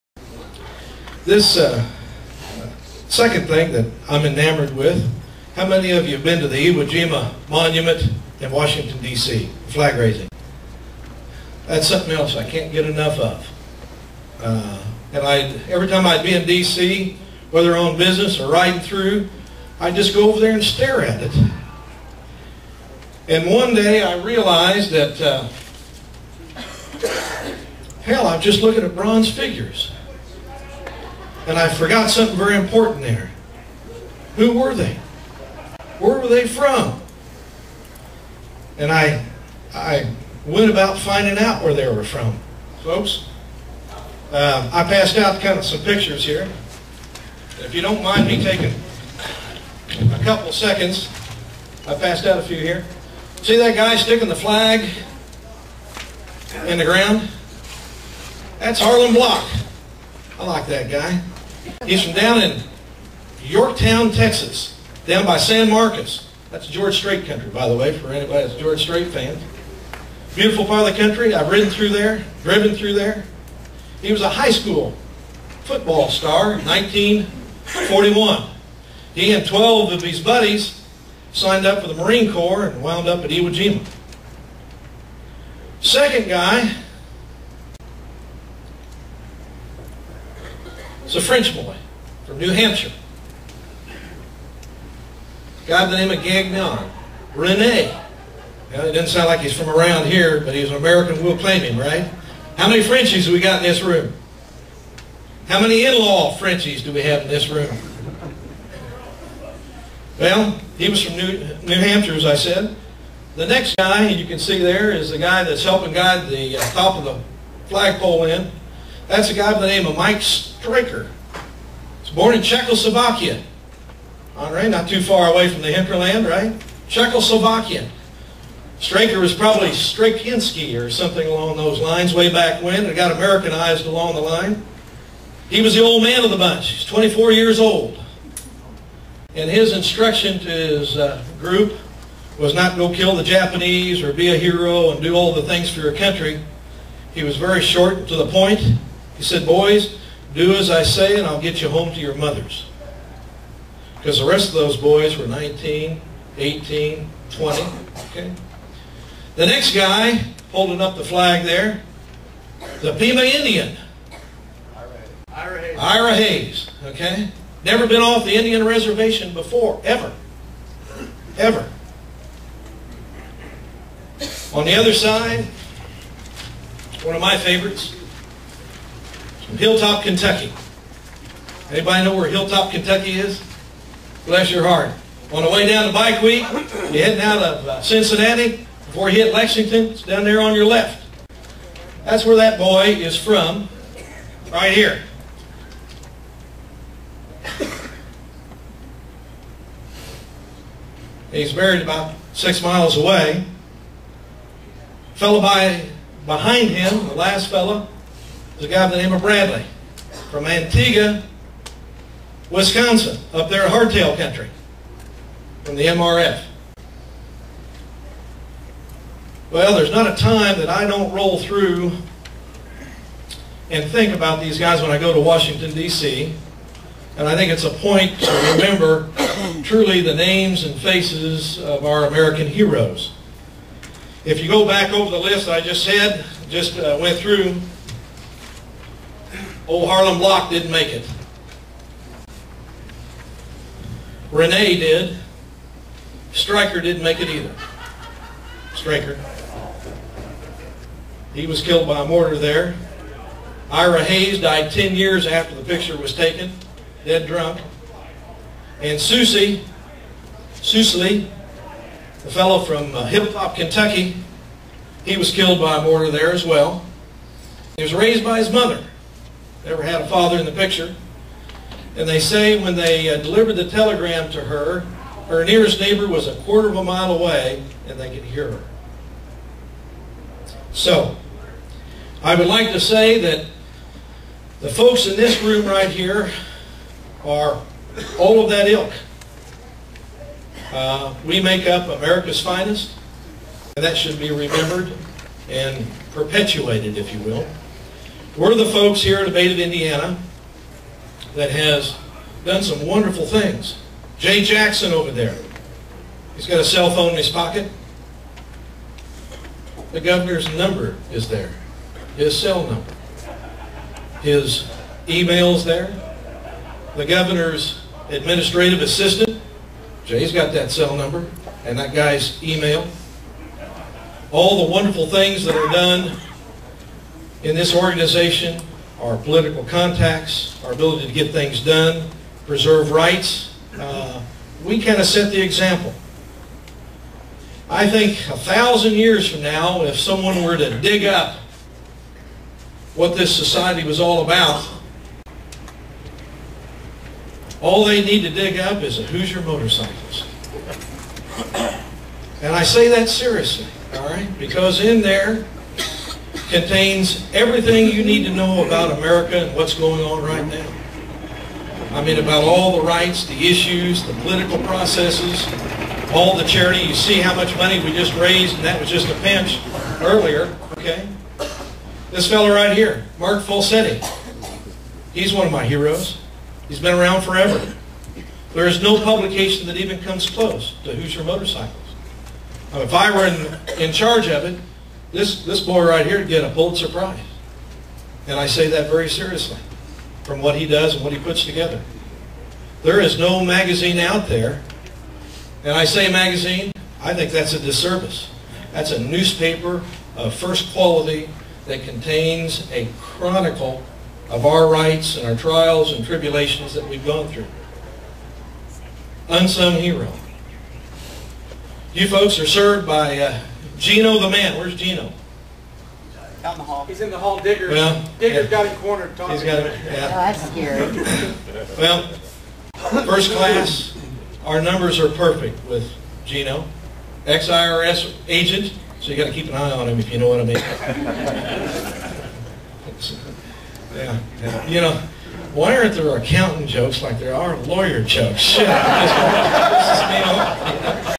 Iwo Jima Monument.wmv
ABATE of Indiana has an annual Leadership Seminar for all of it's officers. After the opening it is down to business, or as best as can be done. It would be a boring seminar if it was only facts and figures - simple examples to bring out a point are very important for this event and are used whenever possible.